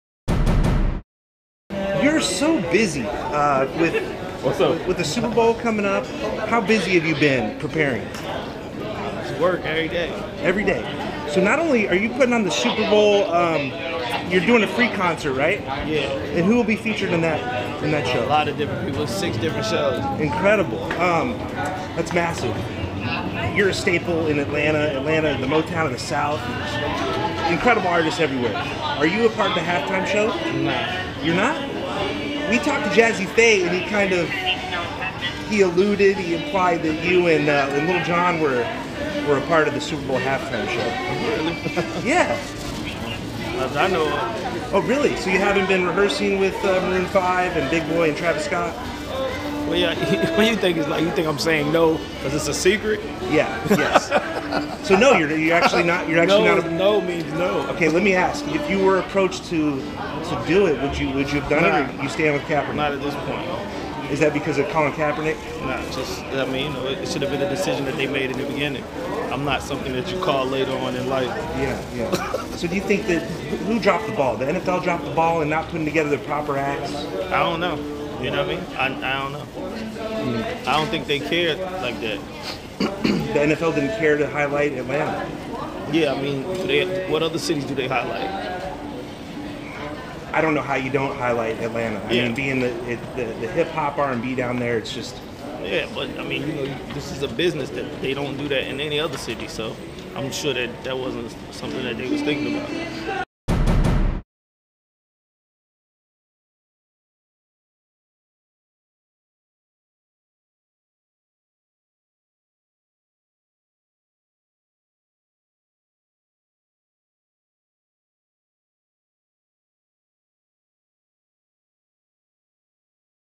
Jermaine Dupri Denies Doing Super Bowl Halftime Show, 'No Means No' | TMZ Sports
Jermaine Dupri says super producer Jazzy Pha is WRONG -- he never signed on to participate in the Super Bowl halftime show ... but it has nothing to do with Colin Kaepernick.